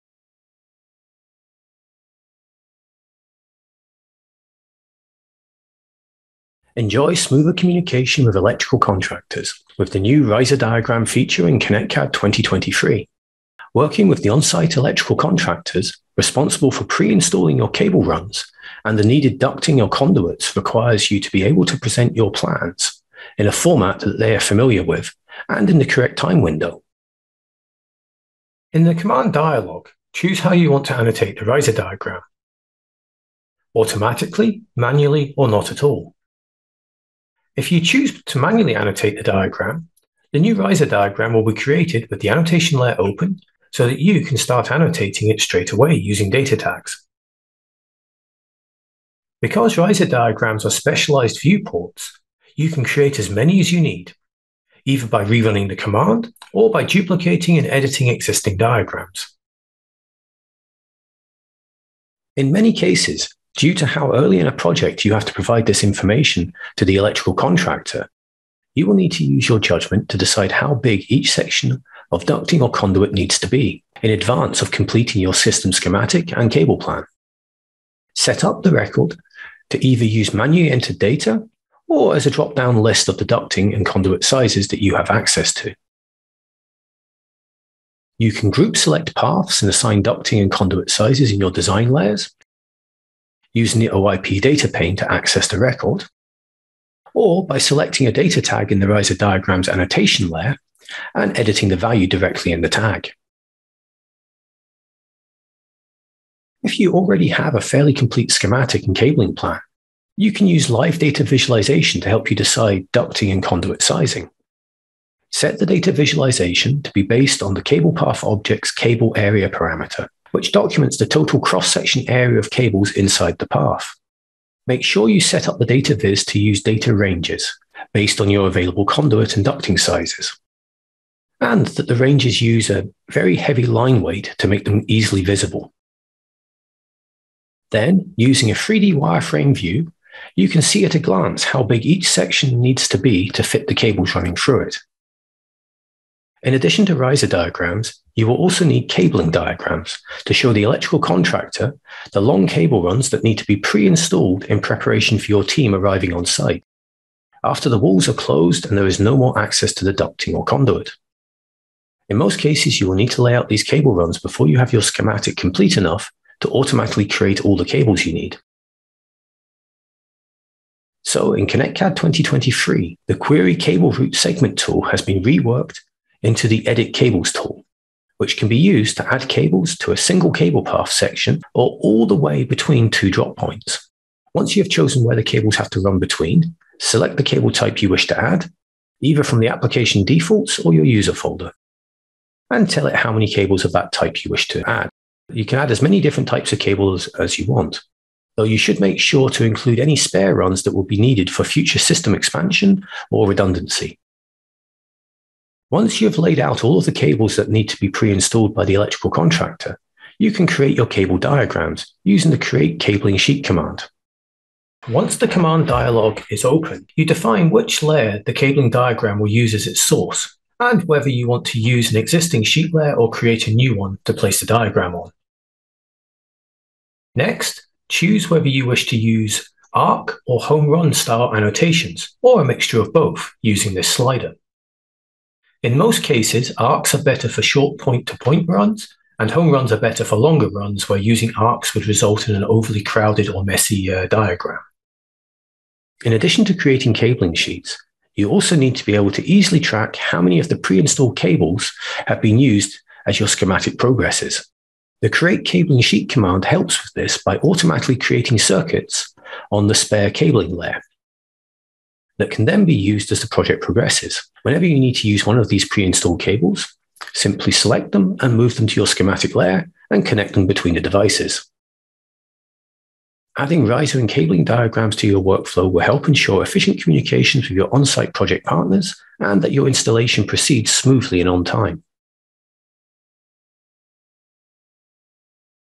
ConnectCAD Riser Diagrams
Updates to the ConnectCAD Cable tool are designed to provide a faster and more accurate way to create cable riser diagrams and reports. The new ability to generate cable riser diagrams will allow you to produce documentation familiar to electrical contractors, so you can get faster responses to on-site changes and quickly evaluate costs.